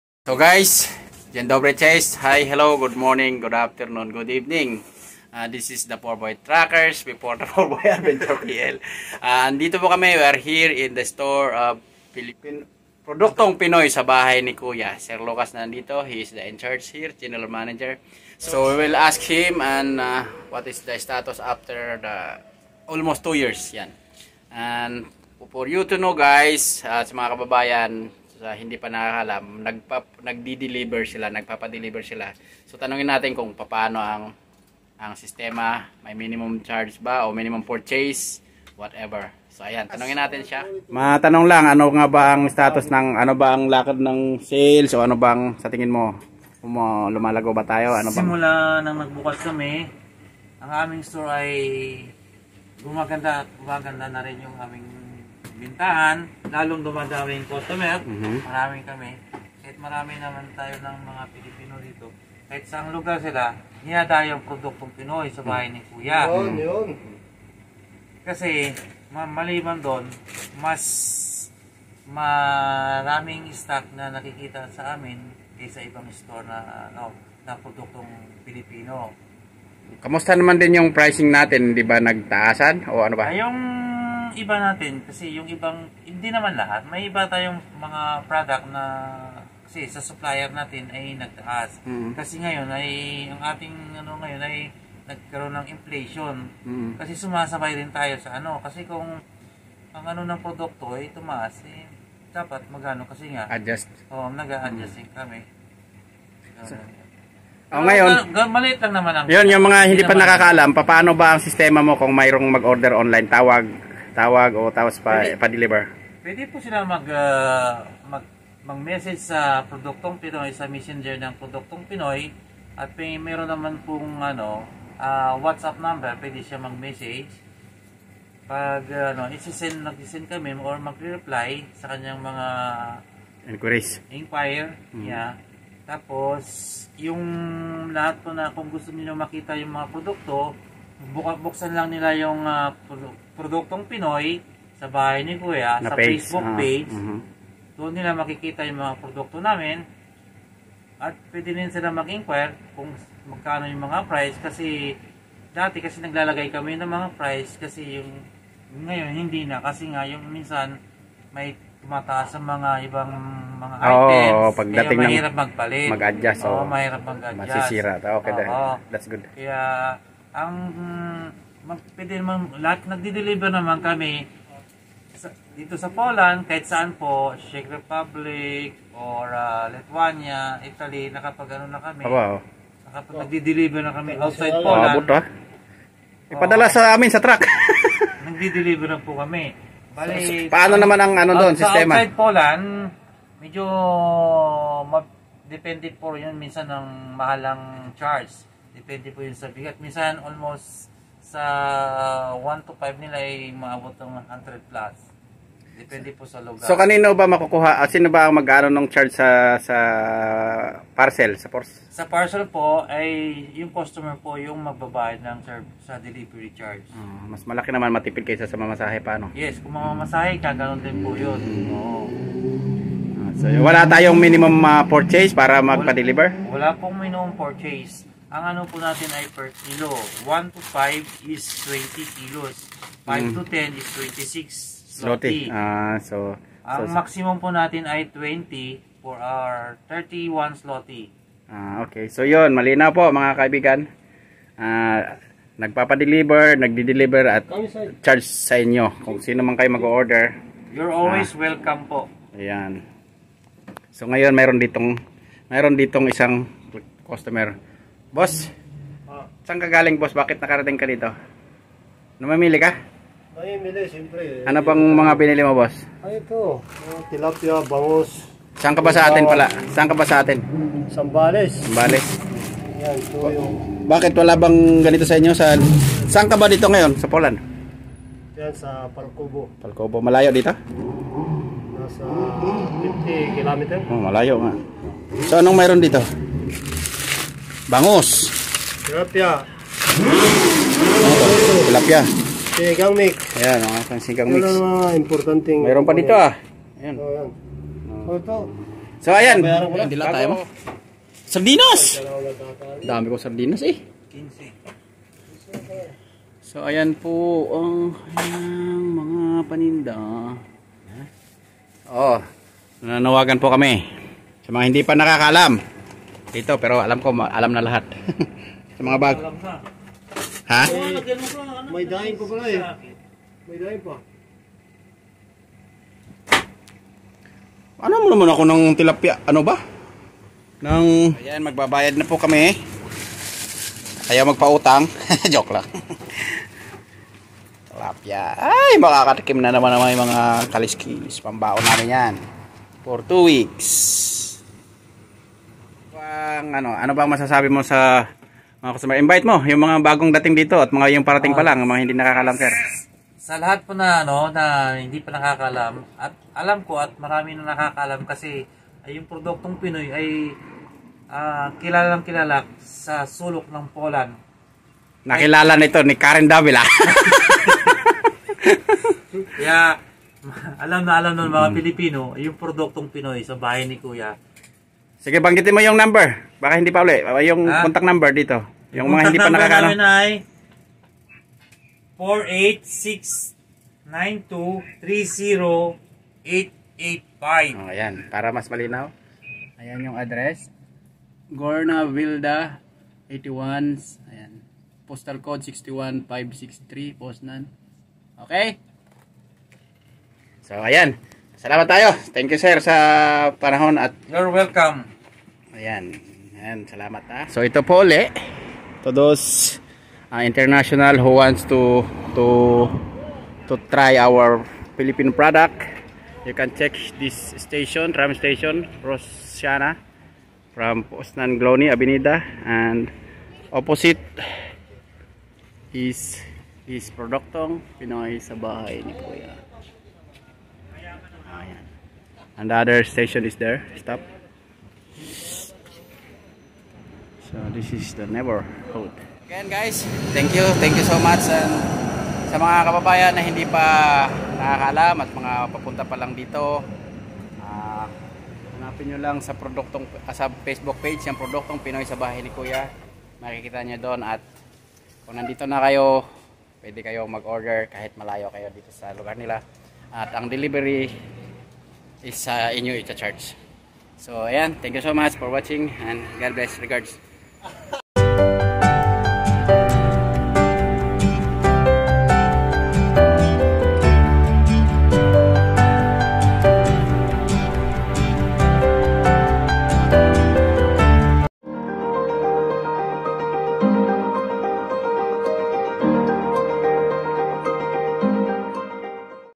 Visited Produktong Pinoy Store in Poznan Poland
Visited Produktong Pinoy Store in Poznan and Bought some Pinoy Products and had a short talk to Mr.Lucasz Andia,He explained about how to order Online or by Phone and to be dilever in your doorstep around Poland and Neighboring countries.
The delivery cost will be change to the customer of course.